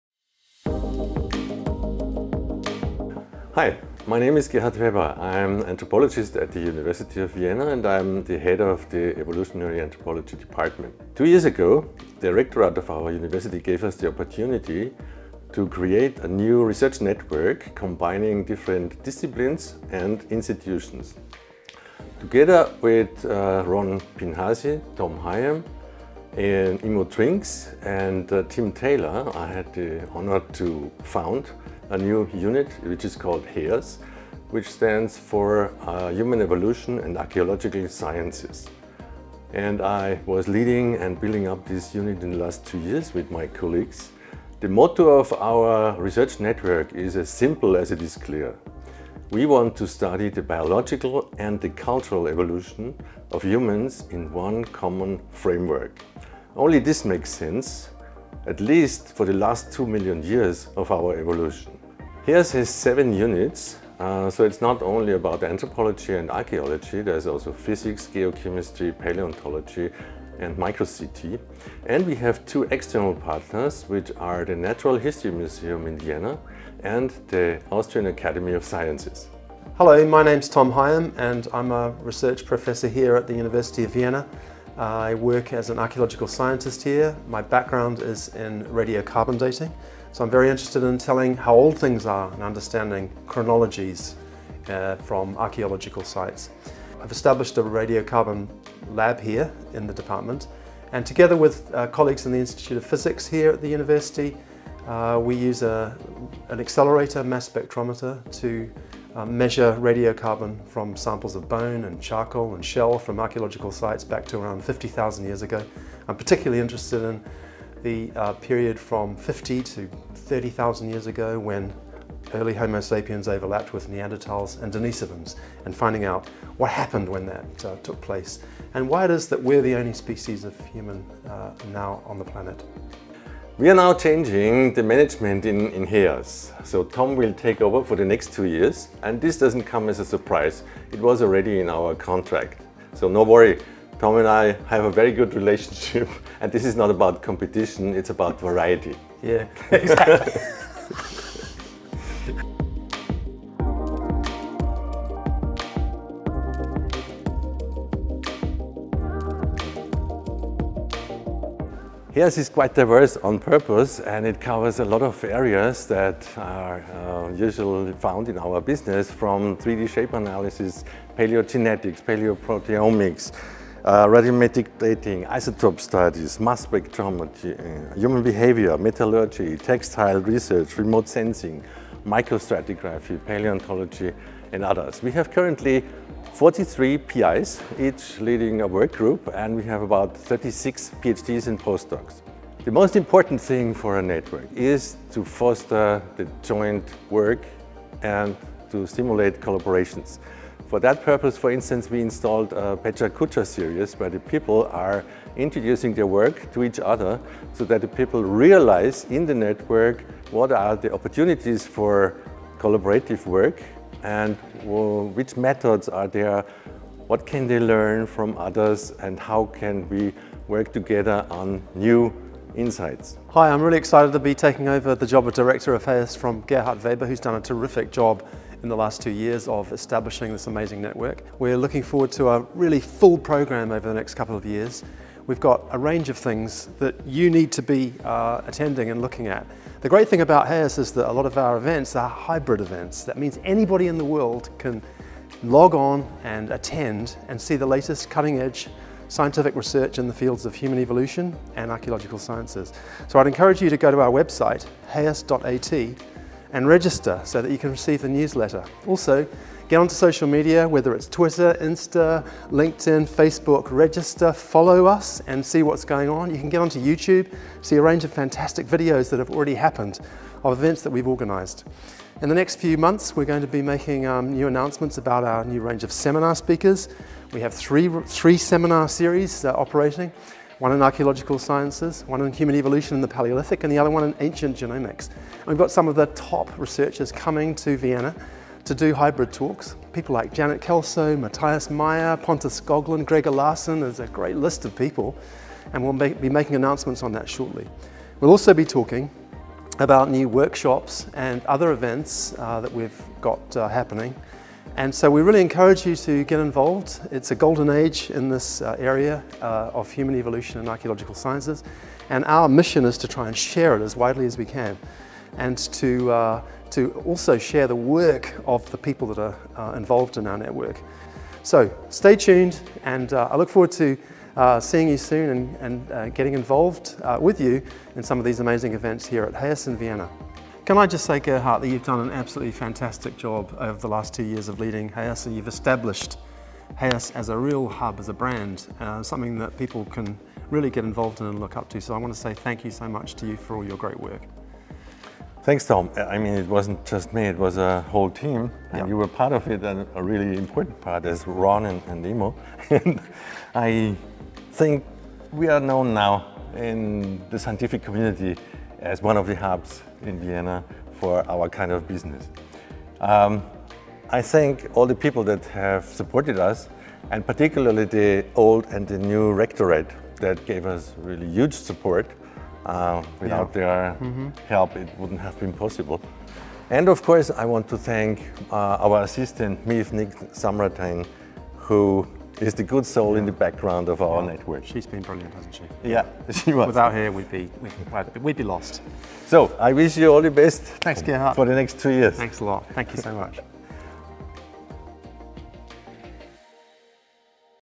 Tom Higham takes over as the Head of HEAS
On the 1st September 2023, Tom Higham took over the leadership of HEAS from Gerhard Weber. Here they talk about what has been achieved so far and what the plans are for the future.